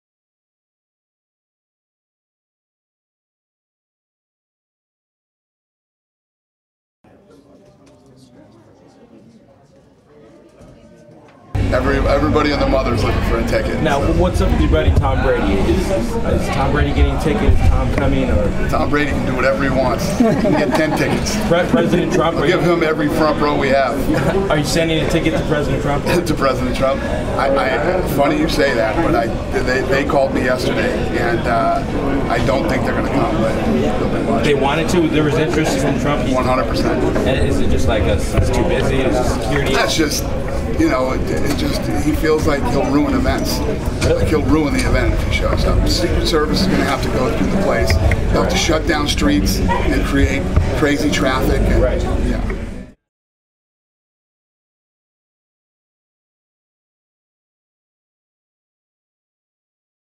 Dana White Says Tom Brady Can Have MayMac Tix; Doesn’t Think Trump Will Come
MMA H.E.A.T. was backstage at the Staples Center in Los Angeles, California for the first stop of the MayMacWorldTour where UFC President Dana White talks about how many Mayweather/McGregor tickets New England Patriots’ QB Tom Brady can have. He goes on to discuss the call he received from Donald Trump’s people and let’s you know whether he thinks Trump will come to the fight. Floyd Mayweather (49-0) vs UFC Lightweight Champion Conor McGregor takes place August 26, 2017 inside the T-Mobile Arena in Las Vegas, Nevada.